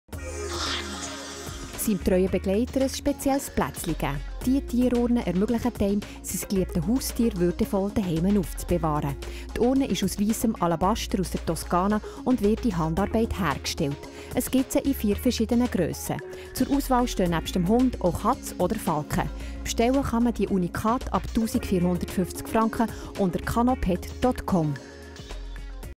Pet Urns from Kanopet
Kanopet produces exclusive pet urns for dignified storage at home. The burial vessels in the form of ancient Egyptian canopic jars grant the deceased pet a dignified return home. The elegant urns are crafted by hand from quality alabaster in Volterra, Tuscany. Painstaking care and attention to detail makes each urn utterly unique. Your loyal companion deserves nothing less.